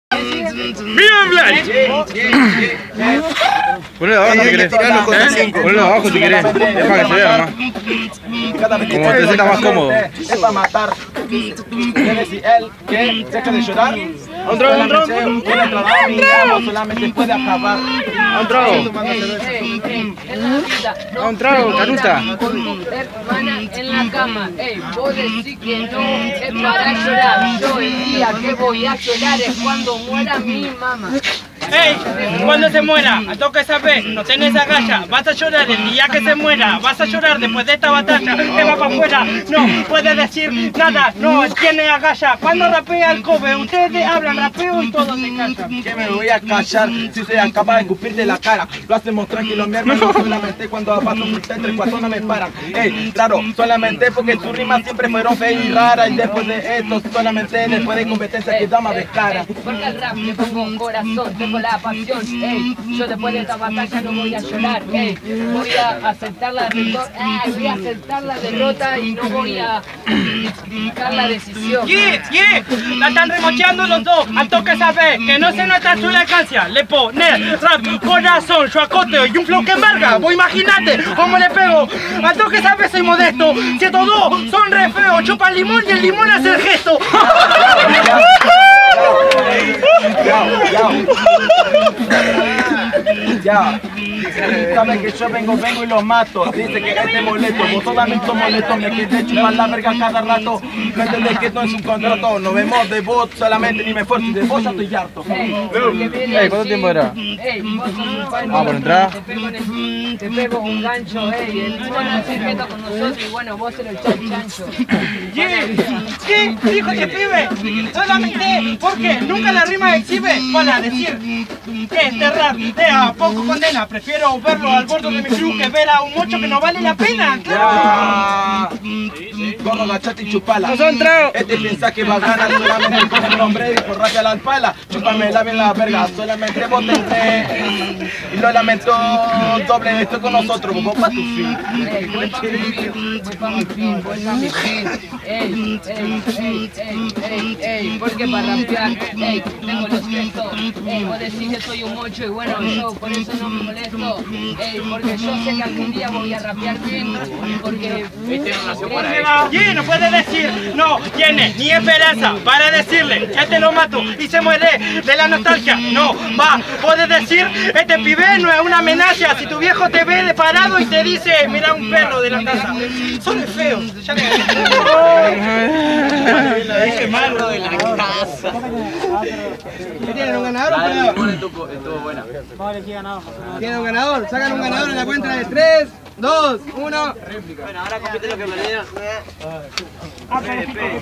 MIXER vs COBE vs DOBLE F - 4TOS - RAP CLIQUE 35° - 10° FECHA
Bueno los esperamos a todos el 20 de Diciembre en lo que va a ser la decima fecha de la rap clique en la cual sera por puntos los 7 primeros en la tabla al finalizar el torneo clasificaran a la 2da edicion con micro de rap clique, el que termine primero se llevara un premio sorpresa, espero que puedan caer todos

MODALIDADES:

8vo: 4 x 4 1:20 (sin tematica)

4to: 40 ida y ida (con tematica)

SEMI: 8 x 8 3min (sin tematica)

FINAL: 1min ida y vuelta (con tematica)

GANADOR: COBE